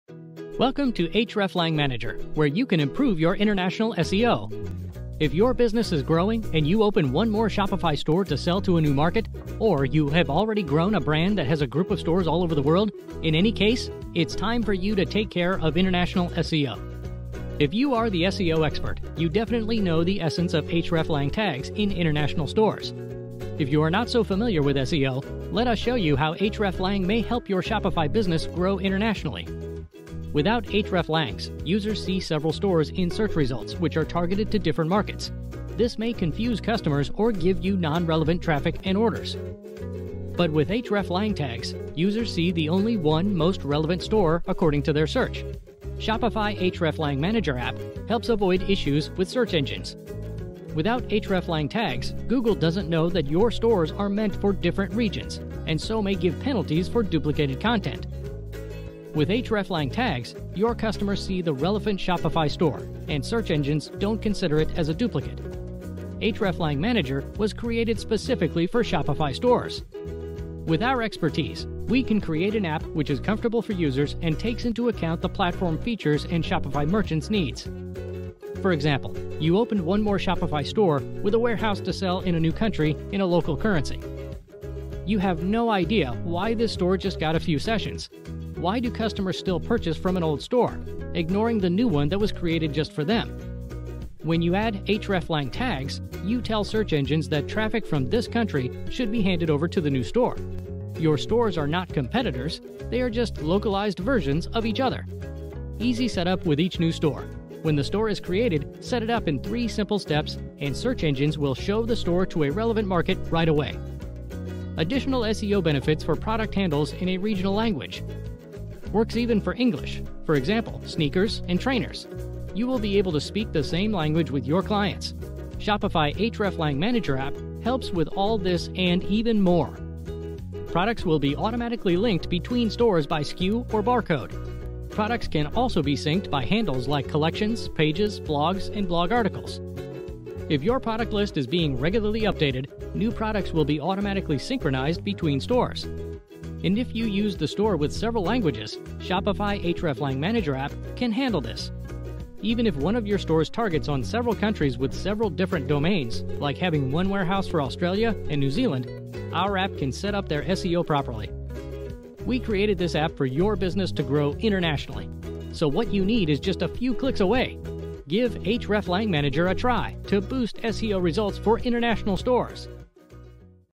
Improve SEO with hreflang tags for Shopify multilingual stores from Hreflang Manager App
Welcome to Hreflang Manager, where you can improve your International SEO.

Your business grows and you open one more Shopify store to sell to a new market. Or, you are already a big-name brand that has a group of stores all over the world.

In any case, it’s time for you to take care of International SEO.
If you are the SEO expert, you definitely know the essence of hreflangs tags in international stores!

Without hreflangs, users see several stores in search results that are targeted to different markets. This may confuse customers or give you non-relevant traffic and orders. But with hreflang tags, users see the only one, most relevant store:

1. Without hreflang tags, Google doesn’t know that your stores are meant for different regions and so may give penalties for duplicated content.

With hreflang tags, your customer sees the relevant Shopify store, and search engines don’t consider it as a duplicate.

For example, you opened one more Shopify store with a warehouse to sell in a new country in a local currency. You have no idea, why this store got just a few sessions. Why do customers still purchase from an old store, ignoring the new one that was created just for them?

When you add hreflang tags, you tell search engines that traffic from this country should be handed over to the new store. Your stores are not competitors, they are just localized versions of each other.

2. Easy setup with each new store.
When the store is created, set it up in 3 simple steps and search engines will show the store to a relevant market right away.

3. Additional SEO benefits for product handles in a regional language.
Works even for English, for example, sneakers & trainers. You will be able to speak the same language with your clients.

Shopify Hreflang Manager app helps with all this and even more!

4. Products will be automatically linked between stores by SKU or barcode. Products can also be synced by handles like collections, pages, blogs, and blog articles.

5. If your product list is being regularly updated, new products will be automatically synchronized between stores. And if you use the store with several languages, the Shopify Hreflang Manager app can handle this.

Even if one of your stores targets several countries with several different domains, like having one warehouse for Australia and New Zealand, our app can set up their SEO properly.

We created this app for your business to grow internationally.

Created by GenovaWebArt.
Shopify Solutions since 2012.